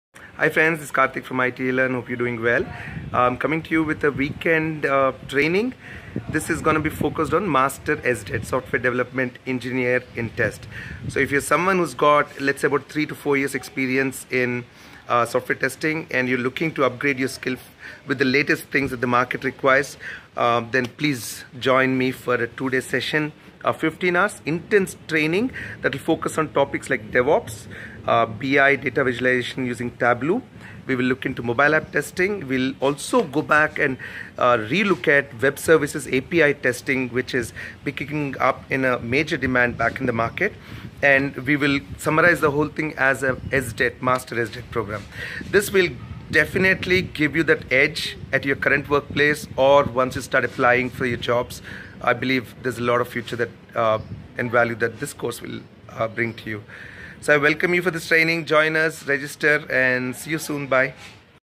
Master SDET Program- Weekend Training
USA- 9th Apr 2018
India - 10th Apr 2018
Timings: 6:30 PM Pacific / 7:00 PM IST

The goal of our weekend training program is to transform your QA / manual testing team to automation. With an aim to produce a competent engineer to design and develop automation testing frameworks.

Enroll in our Professionals Accelerated Transition program and qualify for exclusive job placement opportunities with our recruiting and placement services.

Topics Covered :
- DevOps
- BI / Data Visualization
- SDET
- API, Soap UI Webservices
- Mobile App

For More Info and Free Videos..